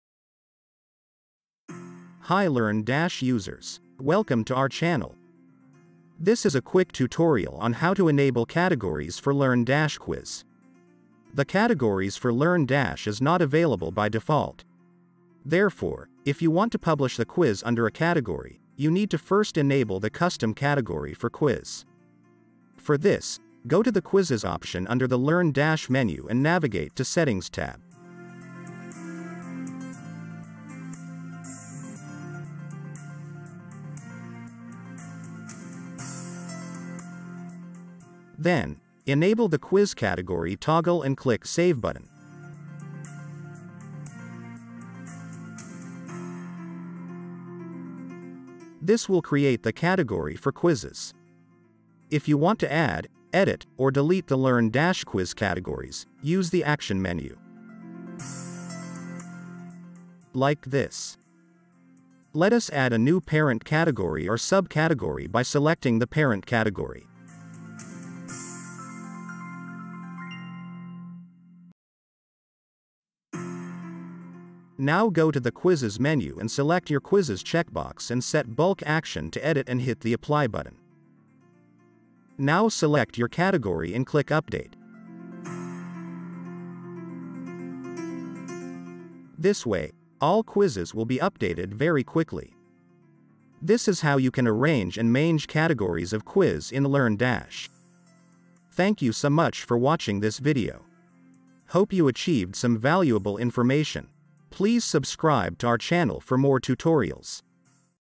How to create and manage categories for Learndash quiz ?
If you wonder how to create categories for quizzes inside LearnDash LMS courses, this video is for you . By default , the categories creation for quizzes are not enabled in Learndash . You need to go to settings area of quiz and enable it . Once you enable, you can use the Action menu on the top right corner of the setting page.

Do you know you can create random quizzes out of categories of quizzes ?